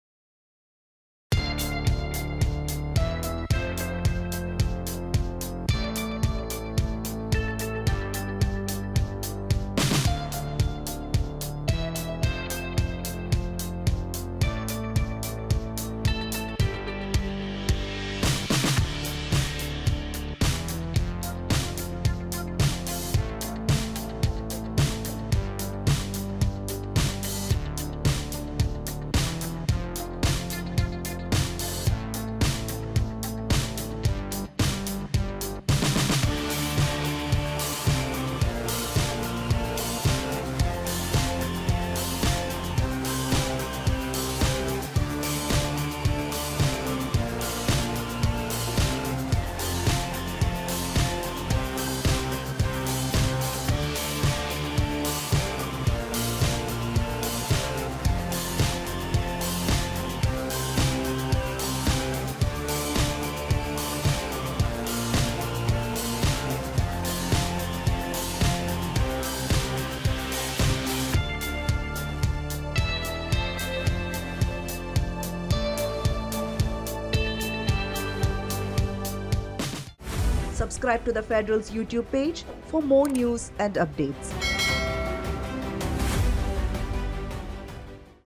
PM Modi's Lok Sabha speech: Key Highlights | The Federal
Prime Minister Narendra Modi launched a scathing attack on the Congress during a debate in the Lok Sabha, sounding the poll bugle for the 2024 polls.